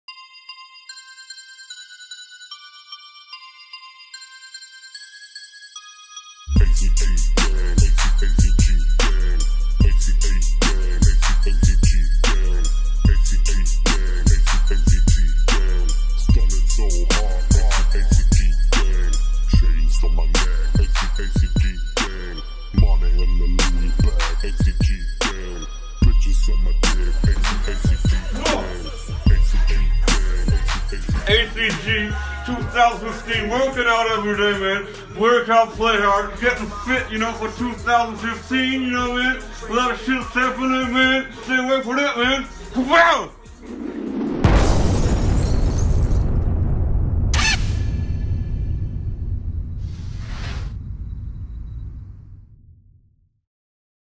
ACG - Workout
Do you want to look like Dick Chainy? Join the exclusive ACG fitness programm! Amazing results in a few weeks!